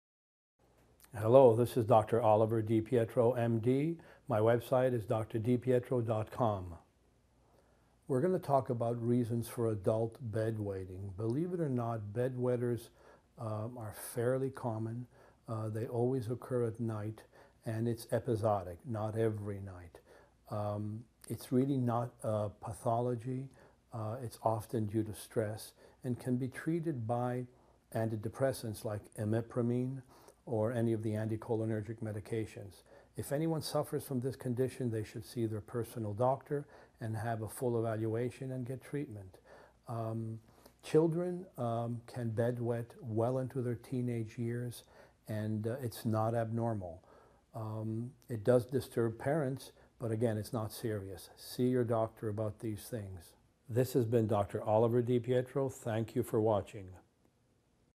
Reasons for Adult Bedwetting
Reasons for Adult Bedwetting. Part of the series: Health Problems. The reasons for adult bedwetting, which is fairly common, include stress. Treat adult bedwetting with antidepressants with help from a medical doctor in this free video on health.